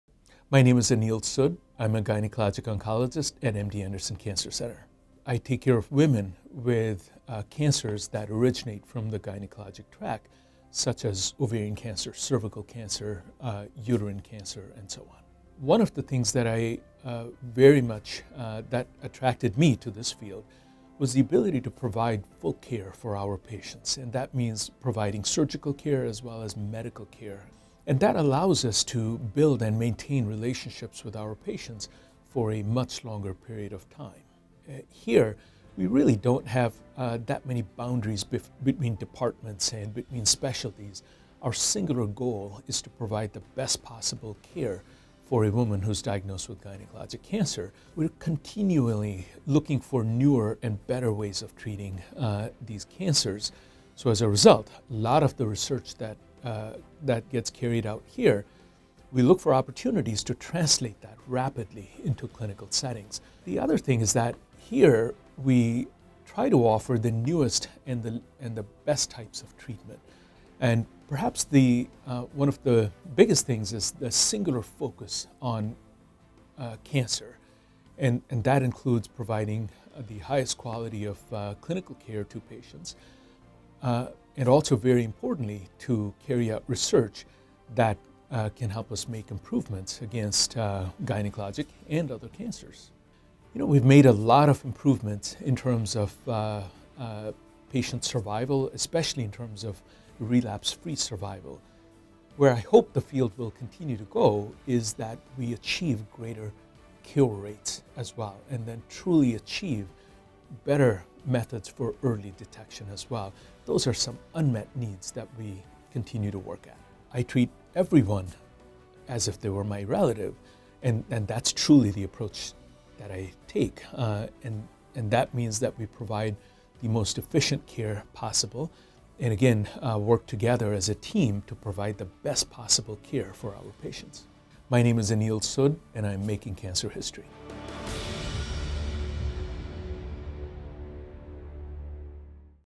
Meet gynecologic oncologist Anil Sood, M.D.
Dr. Anil Sood, M.D., is a professor and Vice Chair for Translational Research in the Departments of Gynecologic Oncology and Cancer Biology, and co-director of the Center for RNA Interference and Non-Coding RNA, at MD Anderson Cancer Center. He is also Director of the multi-disciplinary Blanton-Davis Ovarian Cancer Research Program. Dr. Sood co-leads the Ovarian Cancer Moon Shot Program.

Dr. Sood received his medical degree from the University of North Carolina, Chapel Hill, North Carolina. His research is focused in three main areas: 1) mechanisms of angiogenesis and metastasis in ovarian cancer; 2) effects of neuroendocrine stress hormones on ovarian cancer growth and progression, 3) development of new strategies for systemic in vivo siRNA delivery. Dr. Sood has received major recognition for his research accomplishments including the Hunter Award, the Margaret Greenfield/Carmel Cohen Excellence in Ovarian Cancer Research Prize, and the GCF/Claudia Cohen Research Foundation Prize for Outstanding Gynecologic Cancer Researcher. He is an elected member of the American Society for Clinical Investigation (ASCI), the American Association for the Advancement of Science (AAAS), and the Association of American Physicians (AAP). He was appointed as an American Cancer Society Professor in 2017.